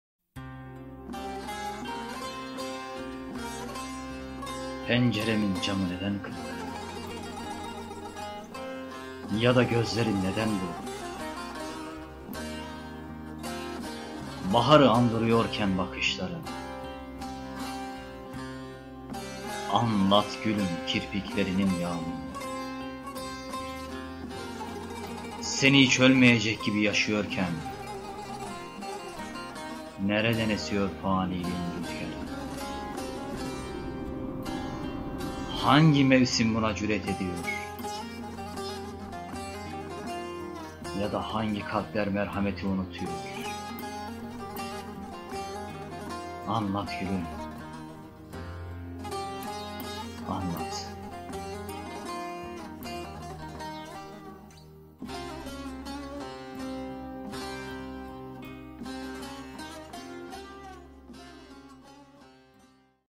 Anlat Şiiri -Yorum:Edebiyatçı Şair
Penceremin camı neden kırık?
Ya da gözlerim neden bulutlu?
Baharı andırıyorken bakışların,
Anlat gülüm kirpiklerinin yağmurunu.
Seni hiç ölmeyecek gibi yaşıyorken,
Nereden esiyor faniliğin rüzgârı.
Hangi mevsim buna cür’et ediyor?
Ya da hangi kalpler merhameti unutuyor?
.🎤Yorum: edebiyatcisair .✍️Şiir: Alıntı .